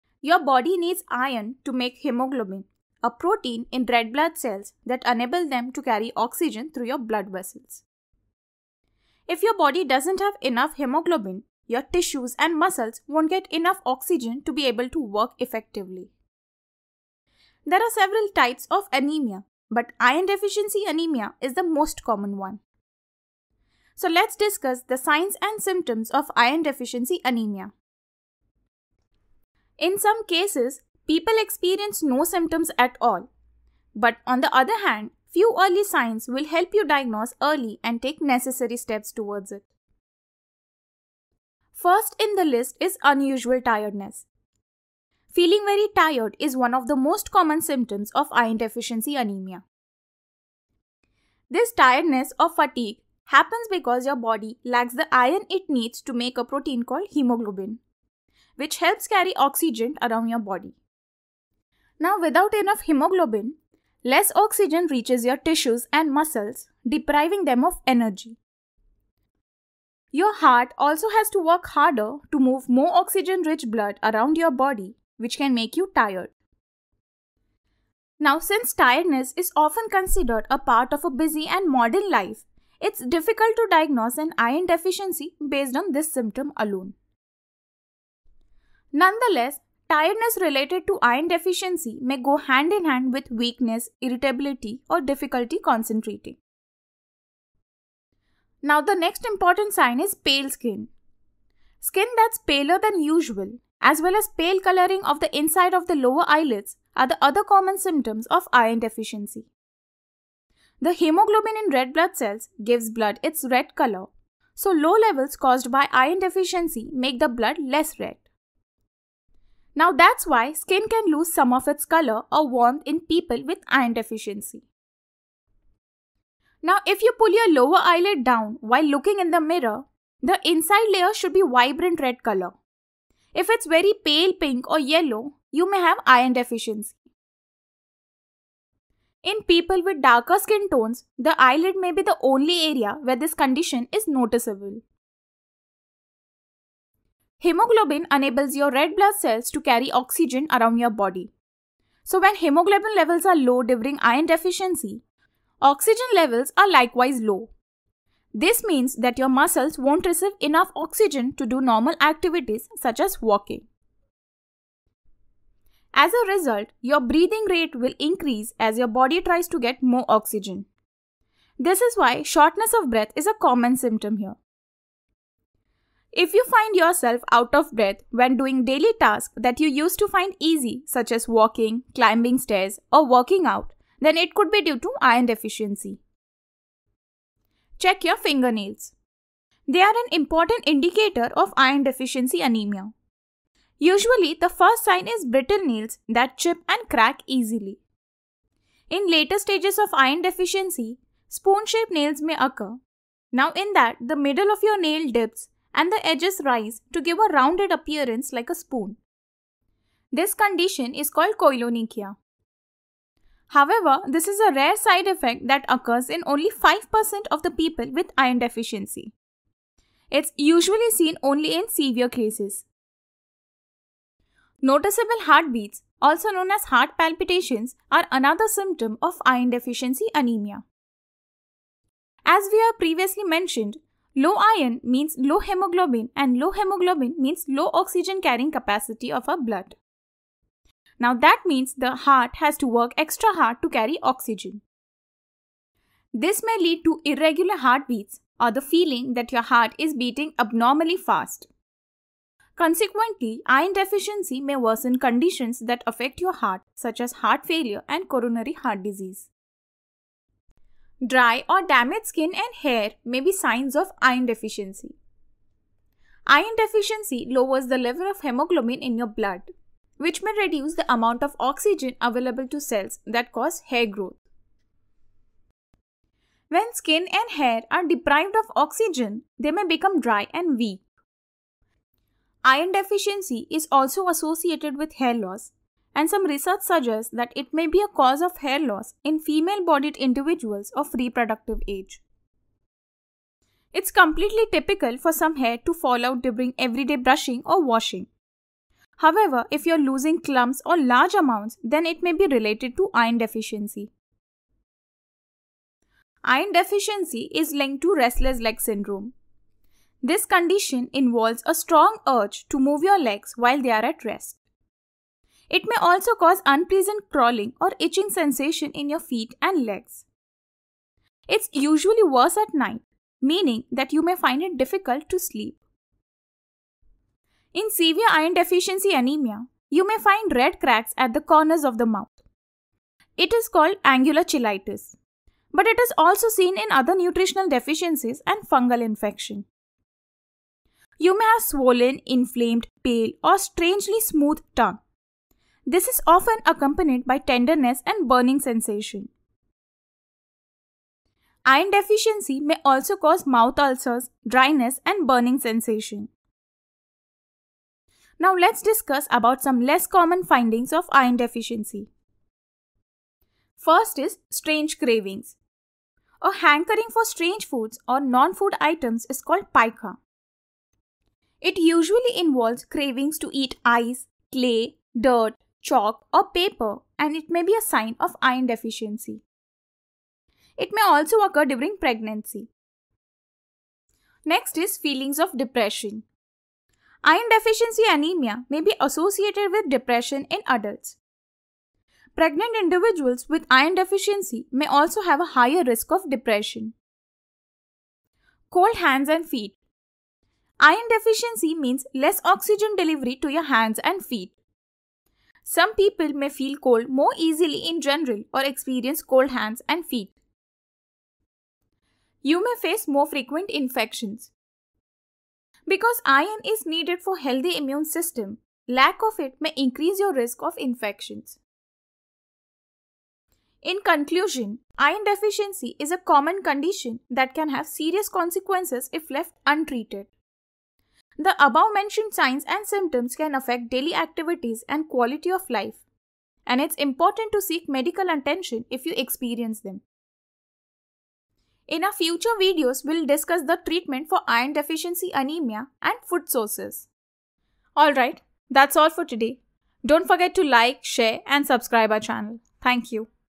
Iron Deficiency Anemia - Signs & Symptoms | Iron Deficiency - Early & Late Signs
Iron Deficiency Anemia - Signs & Symptoms | Iron Deficiency | iron deficiency symptoms | anemia | what is iron deficiency | no more iron deficiency

Iron deficiency anemia signs & Symptoms

There are several types of anemia but iron deficiency anemia is the most common one.

Here in this video we have discussed the signs and symptoms of iron deficiency anemia.

Unusual tiredness:
This tiredness or fatigue happens because your body lacks the iron it needs to make a protein called hemoglobin, which helps carry oxygen around your body.

Pale skin:
Skin that’s paler than usual, as well as pale coloring of the inside of the lower eyelids, are other common symptoms of iron deficiency.

The hemoglobin in red blood cells gives blood its red color, so low levels caused by iron deficiency make the blood less red. That’s why skin can lose some of its color or warmth in people with iron deficiency.

Shortness of breath:
Hemoglobin enables your red blood cells to carry oxygen around your body.
When hemoglobin levels are low during iron deficiency, oxygen levels are likewise low. This means that your muscles won’t receive enough oxygen to do normal activities, such as walking.

As a result, your breathing rate will increase as your body tries to get more oxygen. This is why shortness of breath is a common symptom.

Spoon-shaped fingernails:
Usually, the first sign is brittle nails that chip and crack easily.

In later stages of iron deficiency, spoon-shaped nails may occur, in that the middle of your nail dips and the edges raise to give a rounded appearance like a spoon. This condition is called koilonychia

Heart palpitations:
Low iron means low haemoglobin and low haemoglobin means low oxygen carrying capacity of blood. That means the heart has to work extra hard to carry oxygen.
This may lead to irregular heartbeats or the feeling that your heart is beating abnormally fast.

Dry and damaged hair and skin:
Iron deficiency lowers the level of hemoglobin in your blood, which may reduce the amount of oxygen available to cells that cause hair growth. When skin and hair are deprived of oxygen, they may become dry and weak.

Restless legs:
Iron deficiency is linked to restless leg syndrome.

This condition involves a strong urge to move your legs while they’re at rest. It may also cause unpleasant crawling or itching sensations in your feet and legs.

Oral findings:
In severe iron deficiency anemia you may find red cracks at the corners of the mouth. It is called angular cheilitis.

Iron deficiency may also cause mouth ulcers, dryness and burning sensation

Less common findings of iron deficiency

Strange cravings
Feelings of depression
Cold hands and feet
More frequent infections

In conclusion, iron deficiency is a common condition that can have serious consequences if left untreated. The above mentioned signs & symptoms can affect daily activities and quality of life, and it's important to seek medical attention if you experience them.